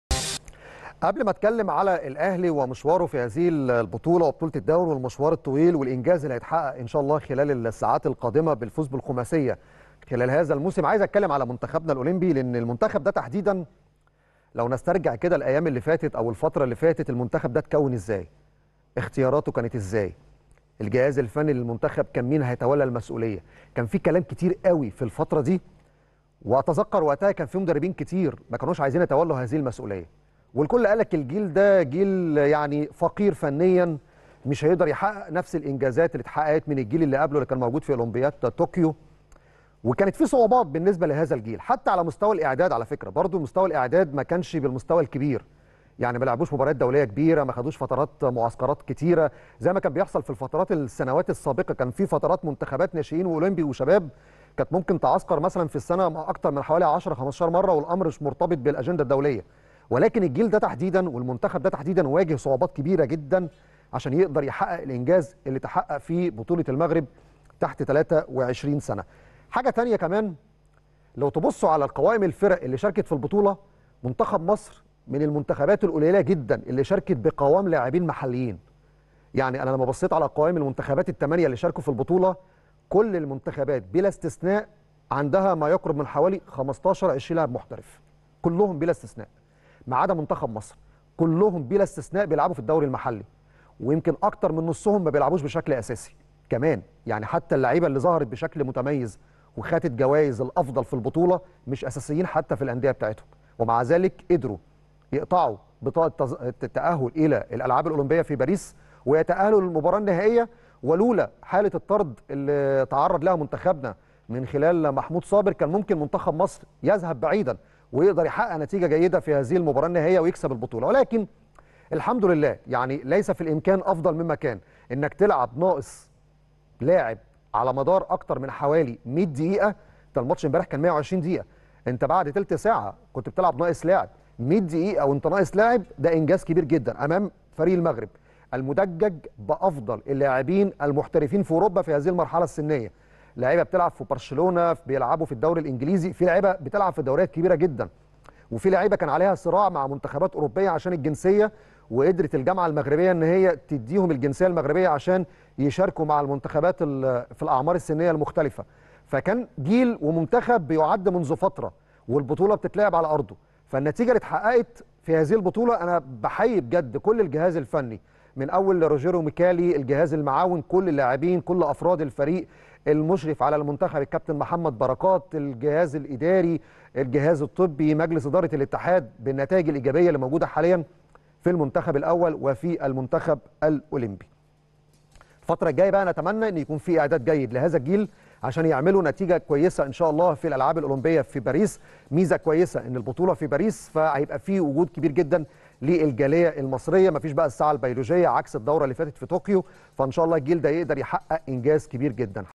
محمد سعيد: منتخبنا الأولمبي قدم إنجاز كبير أمام المغرب .. تحية كبيرة لذلك الجيل الكبير والجهاز الفني
محمد سعيد: منتخبنا الأولمبي قدم إنجاز كبير أمام منتخب المغرب .. تحية كبيرة لذلك الجيل الكبير والجهاز الفني على كل ما قدموه في البطولة